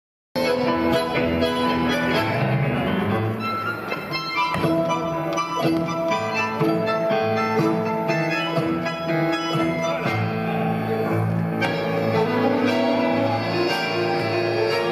Gay tango in Buenos Aires, Argentina
Watching gay tango in Buenos Aires where they have several queer milongas and classes for the LGBTQ community like La Marshall and Queer Tango.

Tango was originally danced between 2 men in the back alleys of Buenos Aires as the voice for the under privileged. This is because there was a shortage of women at the time in the immigrant population, so the only way for men to get them was either via prostitution or to impress them with some sultry dance moves. So the men practised with each other.